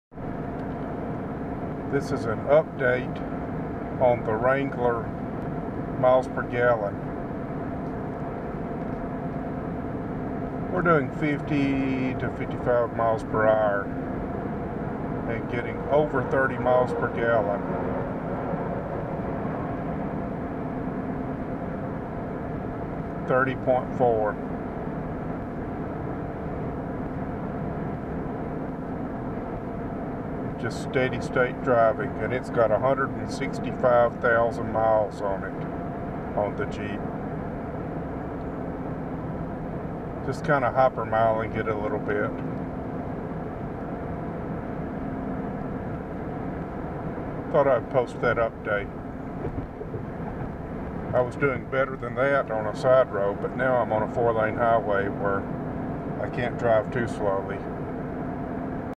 Jeep Wrangler MPG Update
30 mpg on the highway, with 165K miles on the odo!
This is my 2011 Wrangler Unlimited with 3.8l V6, 6 speed transmission, 2.73:1 gears (I really want to put 4.56s in it). Here's how it works: the MPG indicator calculates AVERAGE (not momentary) fuel usage based on fuel flow and speed. I reset it while driving at about 35-40 mph on a paved backroad, because other wise what I had been doing earlier (which was driving through mudholes down by the river, btw) would affect the reported mpg. For awhile I kept it above 50 mpg and even well into the 70s (average remember; not momentary. That is significant) and then settled down to 48 mpg. But then I had to go uphill for a bit, stop and wait for traffic, and accelerate to 50+ mph on the highway. That brought the overall average down to 30 mpg.